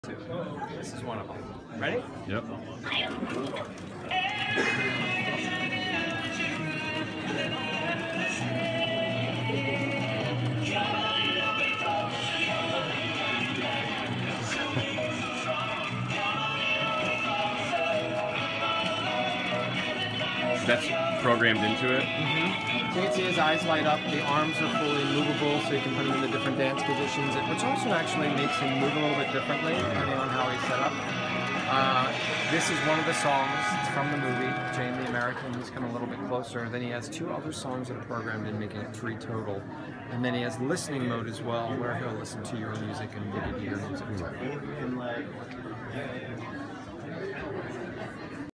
Wall of dancing Baby Groot toy Toyfair 2017 - Guardians of the Galaxy
I couldn't pass up the change to film this wall of dancing baby Groot from Guardians of the Galaxy!